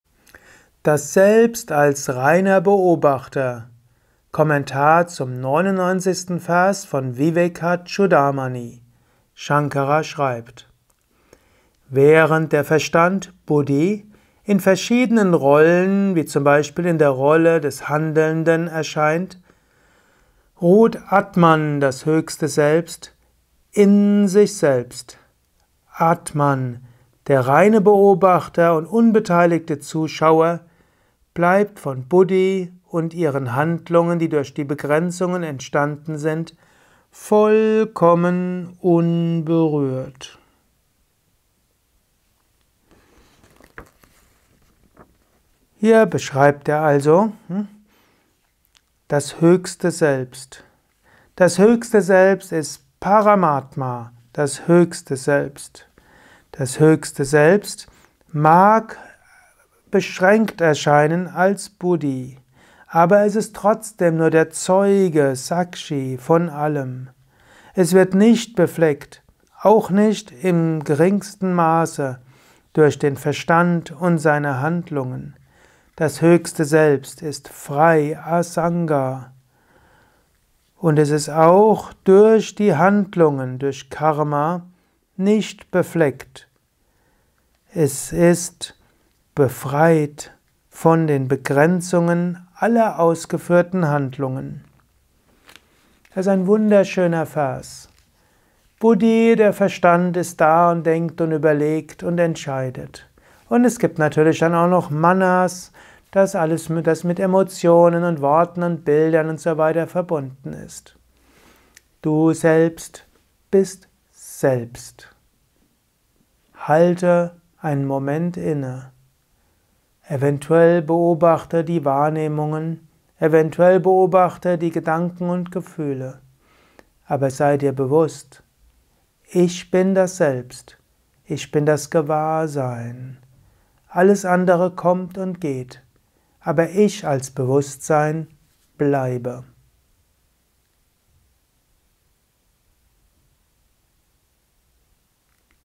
VC0099 Das Selbst als reiner Beobachter - Viveka Chudamani 99.Vers
präsentiert die Viveka Chudamani Reihe.
Sukadev spricht über den 99. Vers des Viveka Chudamani. Dieser Vers lautet:

Während der Verstand (buddhi) in verschiedenen Rollen- wie zum Beispiel in der Rolle des Handelnden – erscheint, verweilt/ strahlt / ruht atman /das Höchste Selbst in seinem eigenen Selbst/ in sich selbst. Atman, der reine Beobachter/unbeteiligter Zuschauer bleibt vo buddhi und deren Handlungen – die durch die Begrenzungen entstanden sind – vollkommen unberührt.